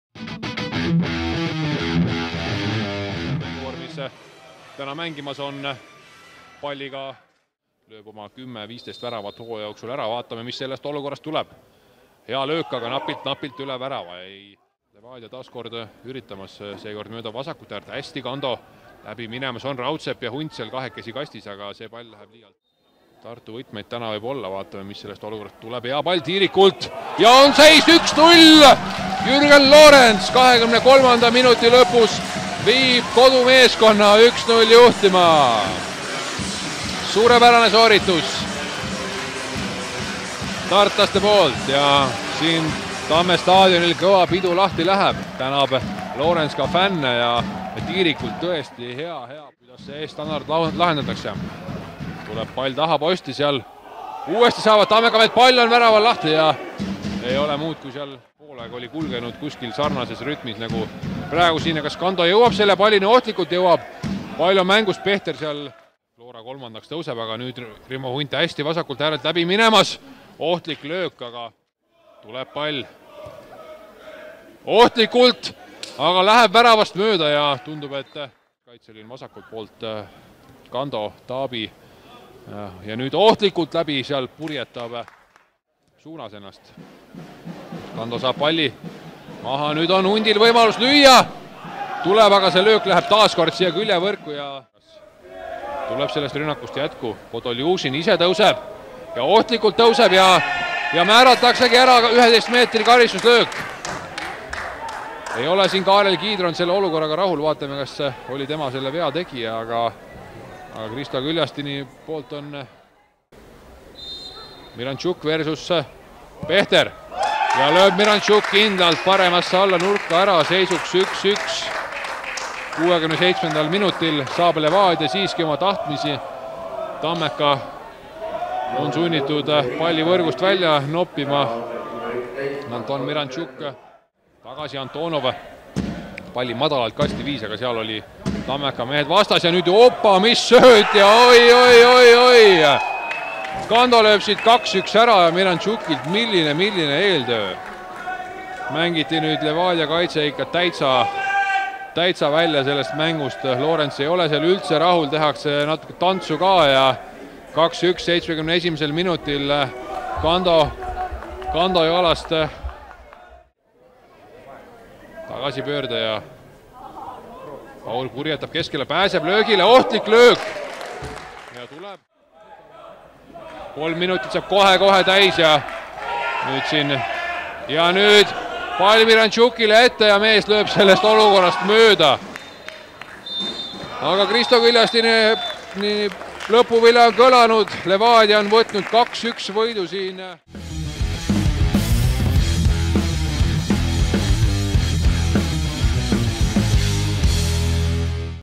24. voor 2016: Tartu JK Tammeka - Tallinna FC Levadia 1:2 (1:0)
Tartu JK Tammeka - Tallinna FC Levadia  1:2 (1:0)
23. Jürgen Lorenz - 67. Anton Mirantšuk, 71. Gando Biala

Hoiatused: 56. Reio Laabus, 67. Silver Grauberg - 11. Kevin Kauber, 73. Ilja Antonov, 80. Sergei Lepmets, 82. Andreas Raudsepp
Mänguolud: +15, pilves vihmane ilm; muruväljak rahuldavas seisukorras.
Pealtvaatajaid: 148
Peakohtunik: Kristo Küljastinen
Abikohtunikud: Dmitri Vinogradov, Risto Eelmaa
Kell 20.00 Tartu Tamme staadionil

Tammeka: Karl Johan Pechter, Kaarel Kiidron, Jürgen Lorenz, Tristan Koskor (85. Erki Mõttus), Tauno Tekko, Martin Miller, Kaspar Paur, Silver Grauberg, Kristjan Tiirik, Reio Laabus, Sander Kapper.
Varus: Andre Paju, Karl Vaabel, Rait Läll, Erki Mõttus, Erko Litvjakov.

Levadia: Sergei Lepmets, Maksim Podholjuzin, Gando Biala, Igor Morozov, Tabi Manga, Marek Kaljumäe, Ilja Antonov, Andreas Raudsepp, Kevin Kauber (58. Jevgeni Kobzar), Alan Gatagov (58. Anton Mirantšuk), Rimo Hunt.
Varus: Priit Pikker, Alger Džumadil, Magnar Vainumäe, Jevgeni Kobzar, Mart Paul Preiman, Nikita Koger, Anton Mirantšuk.

Video: ERR